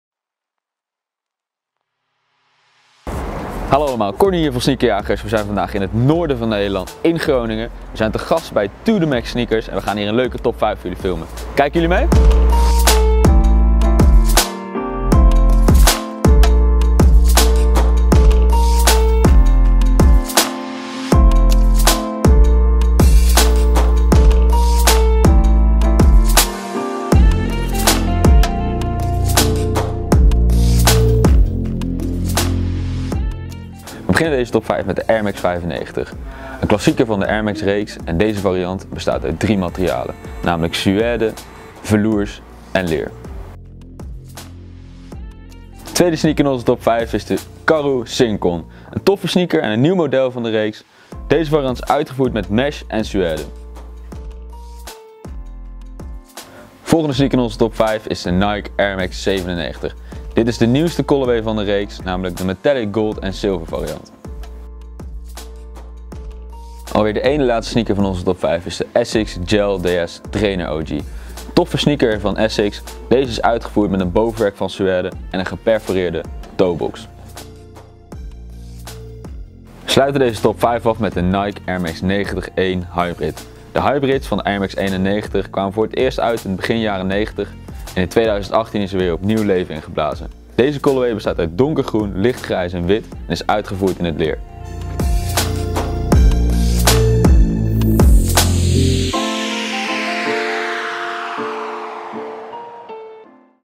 Sneakerjagers Top 5 // 2 The Max Sneakers Groningen
We've visited the sneaker store 2 The Max Sneakers in Groningen. Check out the top 5 sneakers we've selected in the store.

1. Nike Wmns Air Max 95 LX

2. Karhu Synchron Classic

3. Nike Wmns Air Max 97 Special Edition

4. Asics Gel-DS Trainer OG

5. Nike Air Max 90/1